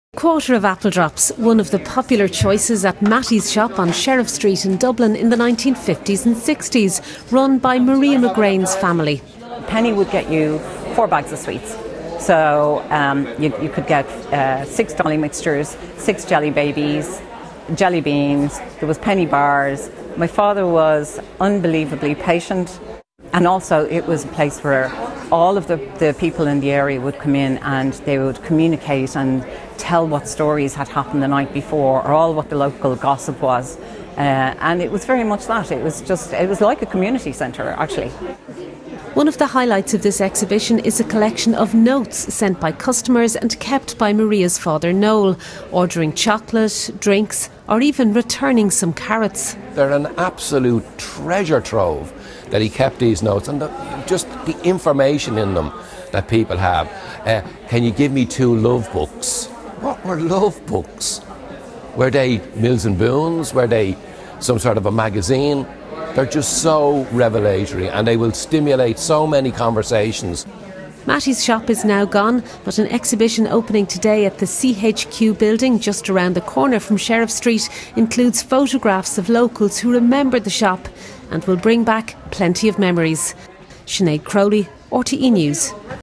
Sweet memories as Dublin confectionery shop recalled
Jars of apple drops, old cigarette boxes and notes asking for everything from soft drinks to 'love books' are among the items at a new exhibition which opens at the CHQ exhibition space in Dublin.

'Pride of Place' was curated by photographer Maria McGrane who grew up on Sheriff Street where her family ran Mattie's sweet shop until the 1970s.